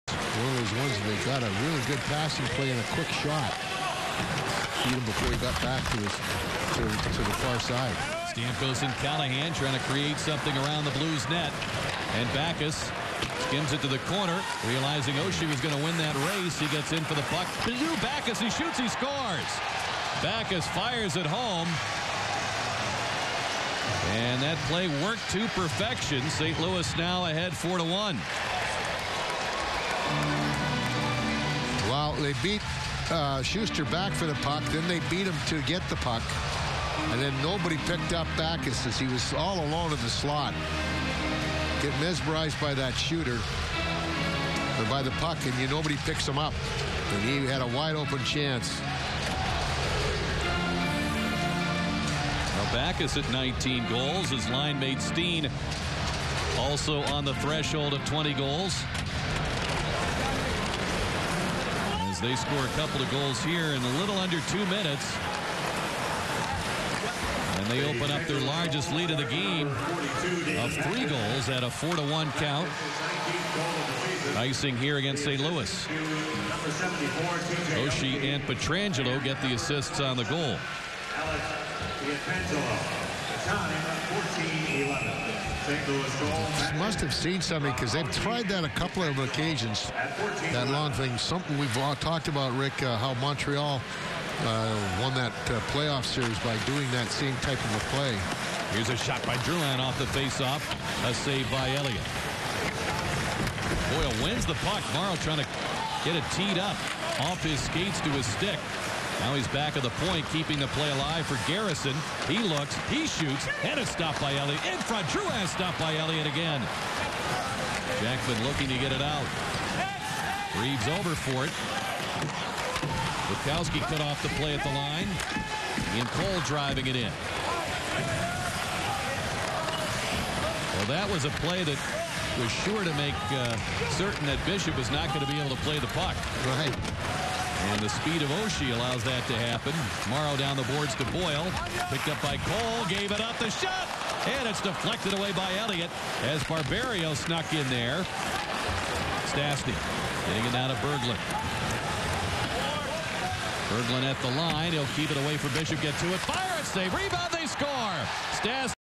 David Backes wrist shot goal 4-1 St. Louis Blues vs TB Lightning Feb 12 2015 NHL
2ND PERIOD

14:11 STL David Backes (19) Wrist shot - ASST: T.J. Oshie (22), Alex Pietrangelo (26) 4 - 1 STL

PBP ?, color ?. NBCSN feed.